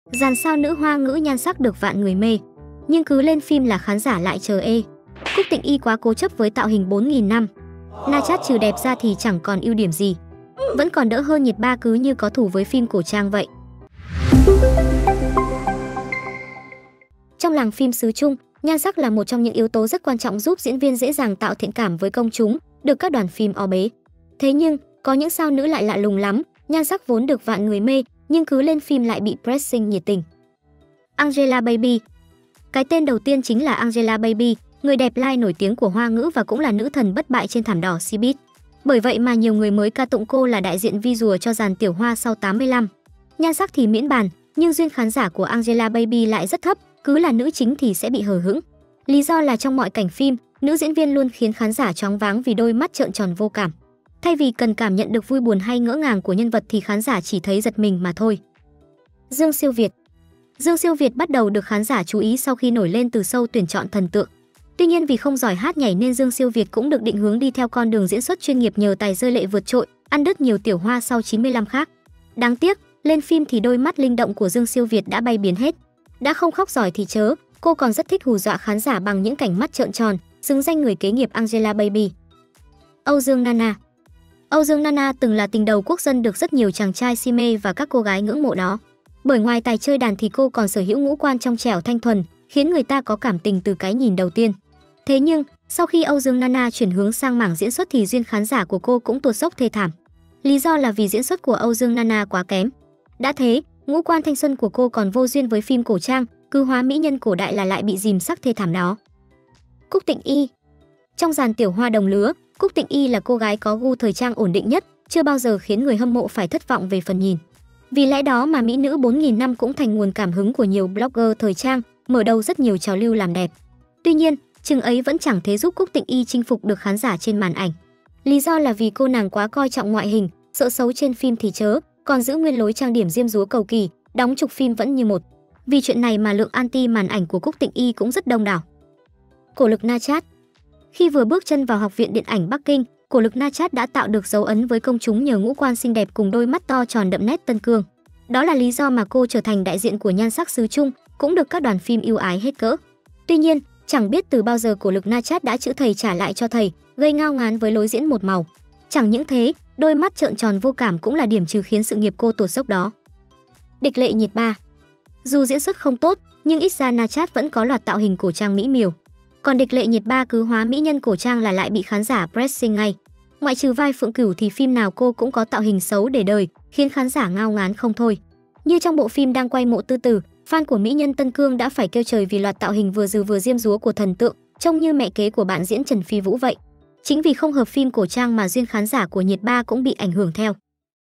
Dàn sao nữ Hoa ngữ nhan sắc được vạn người mê, nhưng cứ lên phim là khán giả lại "chờ ê"
Trong làng phim xứ Trung, nhan sắc là một trong những yếu tố rất quan trọng giúp diễn viên dễ dàng tạo thiện cảm với công chúng, được các đoàn phim o bế. Thế nhưng, có những sao nữ lại lạ lùng lắm, nhan sắc vốn được vạn người mê, nhưng cứ lên phim lại bị pressing nhiệt tình.